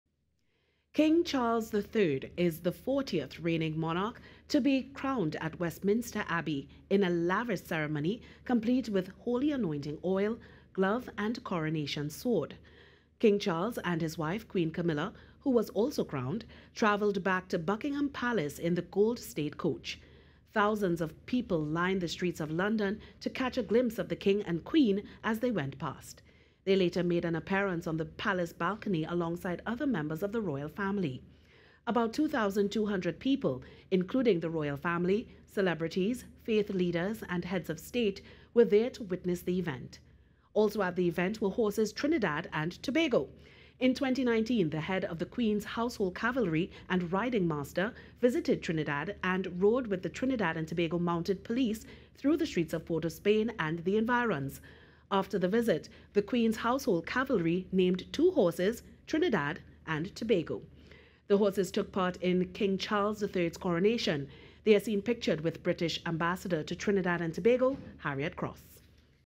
Charles III Crowned
King Charles III is the 40th reigning monarch to be crowned at Westminster Abbey, in a lavish ceremony complete with holy anointing oil, glove and coronation sword